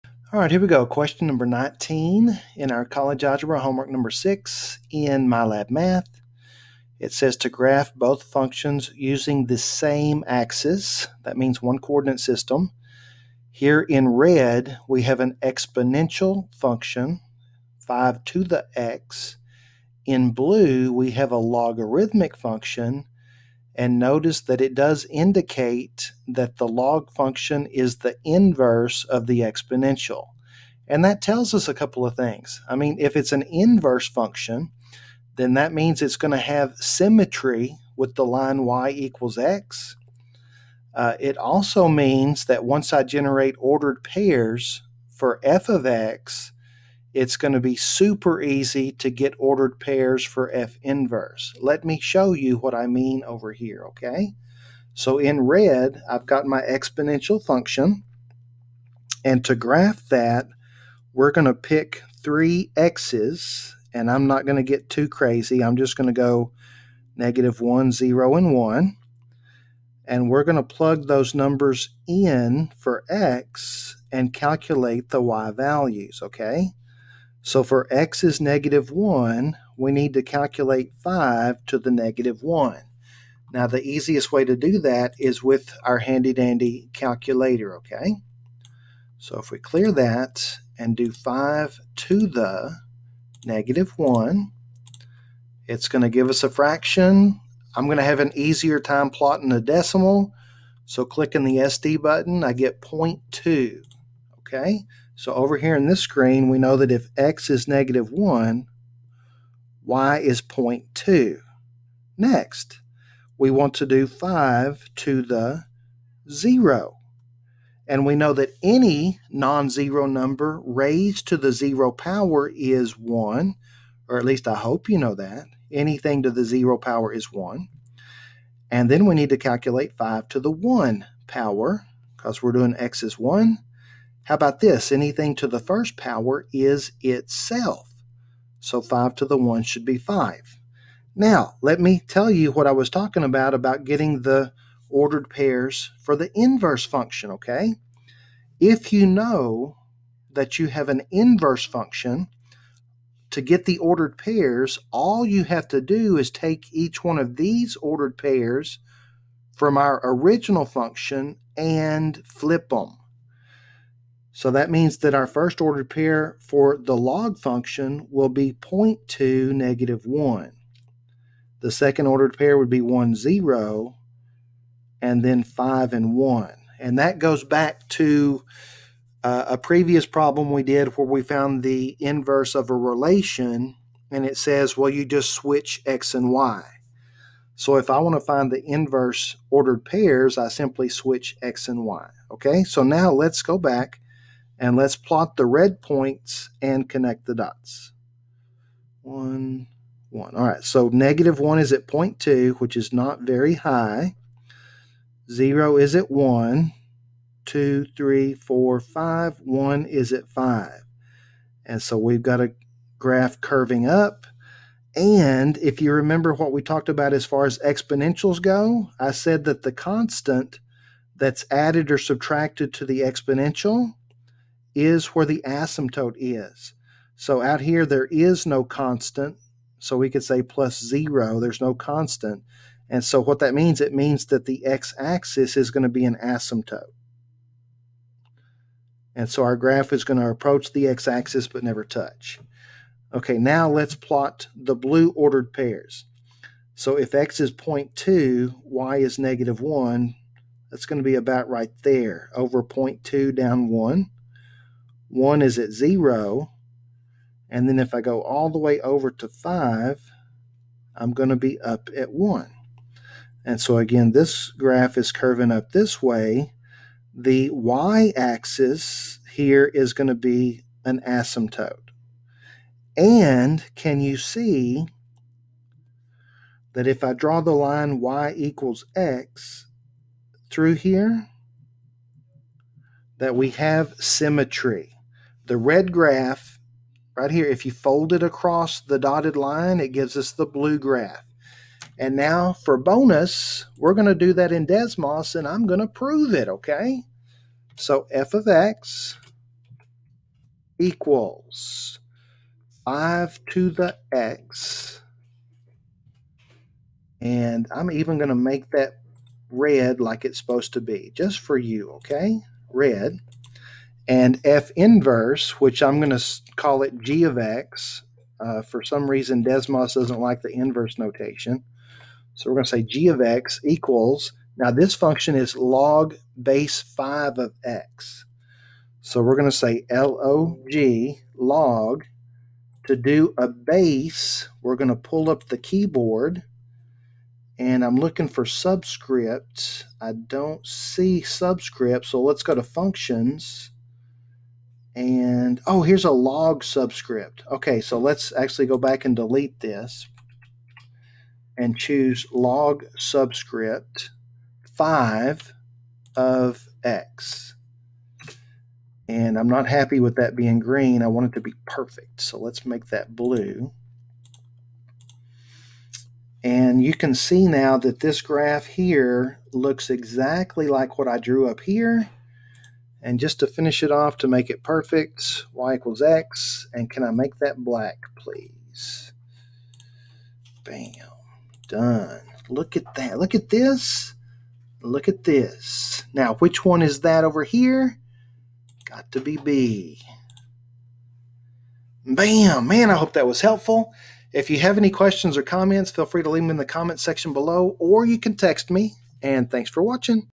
College Algebra - MyLab Math - HW#6: Question #19
Graph an exponential function and its inverse (logarithmic function) in the same coordinate system.